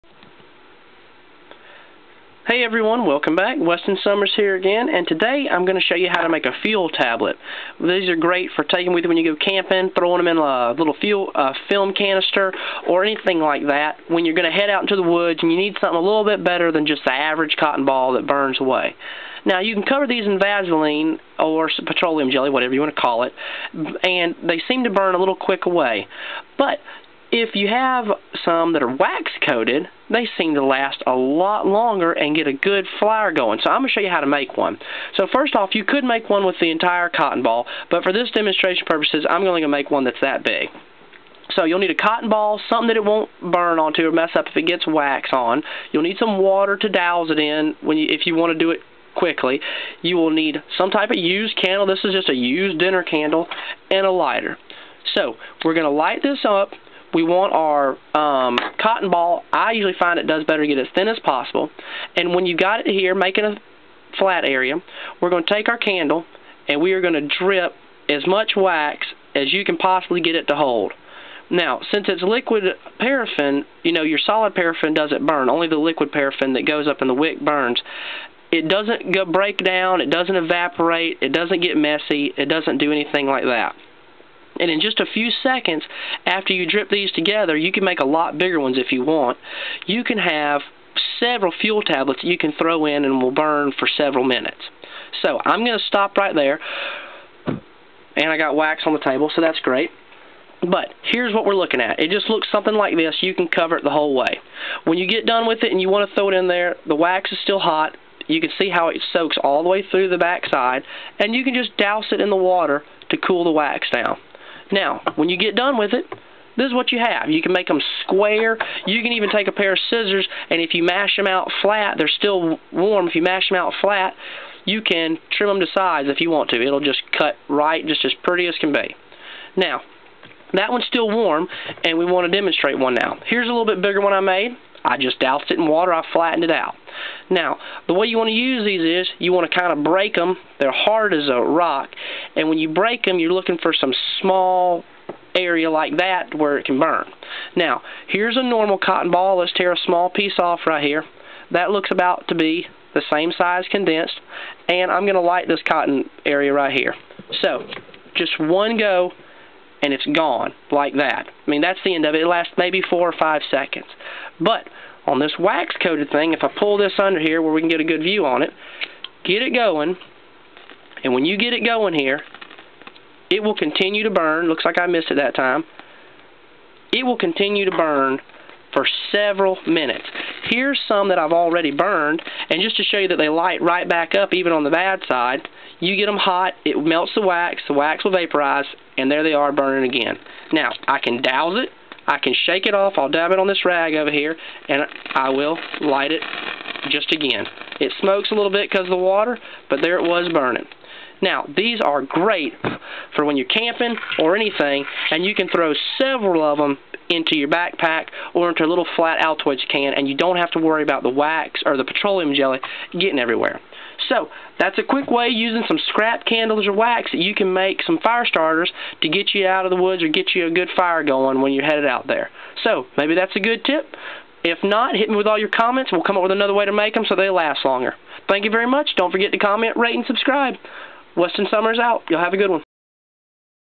Survival Fuel tablets. Cotton and Wax
If you want to make some easy fuel tablets then just check out this video. Cotton balls and wax is all you will need. These fuel tablets never go bad and burn for up to 5min or more depending on size. I usually keep a few stuffed in my bag or pack when we head out. They are great fro damp conditions.